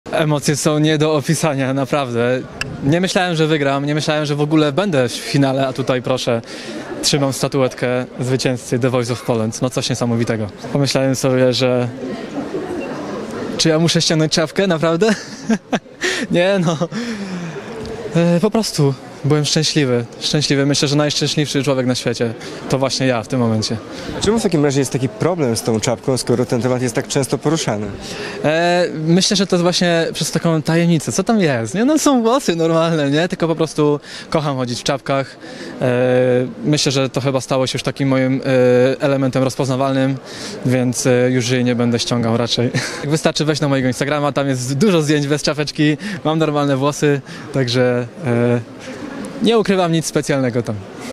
Dominik Dudek w emocjach o WYGRANEJ "The Voice of Poland" w TVP - dlaczego nie zdejmuje czapki?
Wywiad z Dominik Dudek, XIII Finał The Voice of Poland TVP, Warszawa -   19.11.22 r.